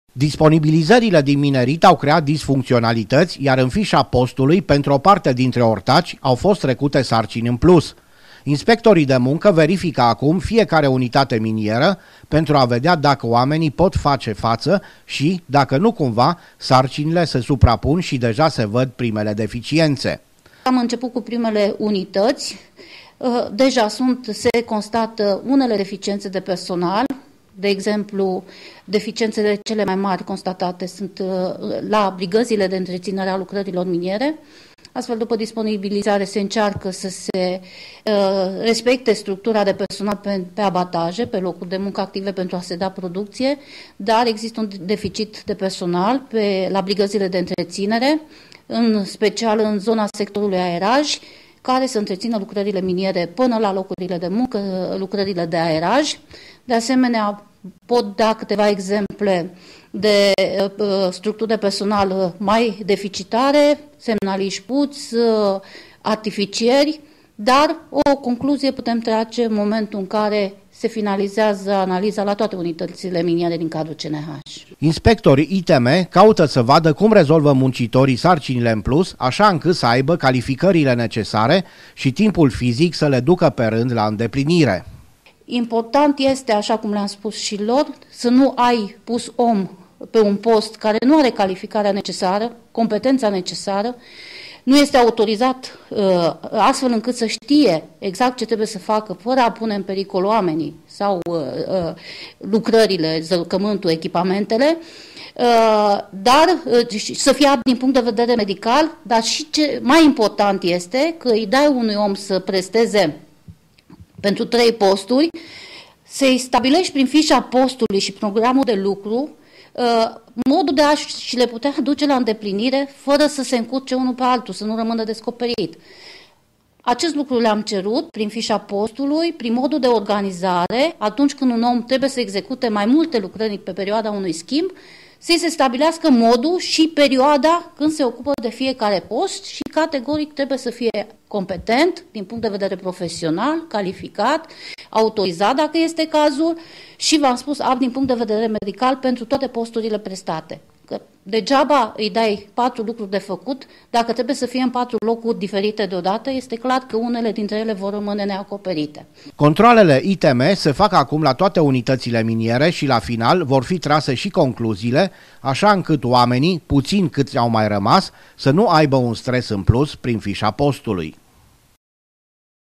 UN OM PE PATRU POSTURI
Disponibilizările din minerit au creat disfuncţionalităţi, iar în fişa postului, pentru o parte dintre ortaci, au fost trecute sarcini în plus. Inspectorii de  muncă verifică acum fiecare unitate minieră, pentru a vedea dacă oamenii pot face faţă şi dacă nu cumva sarcinile se suprapun şi deja se văd primele deficienţe.
Inspectorii ITM caută să vadă cum rezolvă muncitorii sarcinile în plus, aşa încât să aibă calificările necesare şi timpul fizic să le ducă pe rând la îndeplinire.
Controalele ITM se fac acum la toate unităţile miniere şi la final vor fi trase şi concluziile,aşa încât oamenii, puţini cât au mai rămas, să nu aibă un stres în plus, prin fişa postului.